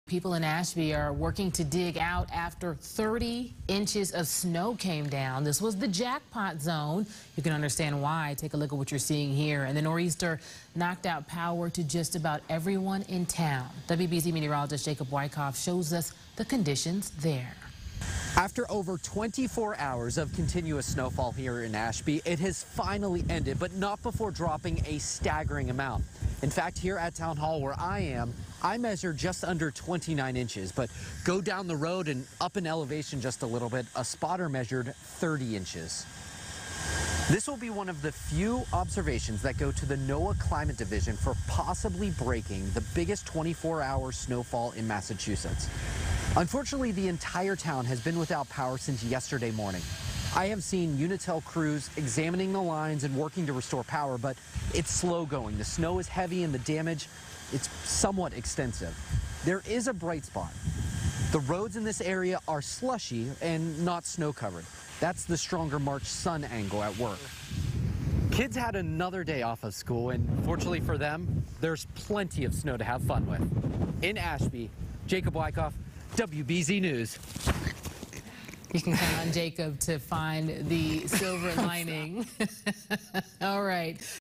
Ashby still without power a day after getting 30 inches of snow in nor'easter
WBZ TV's Jacob Wycoff reports.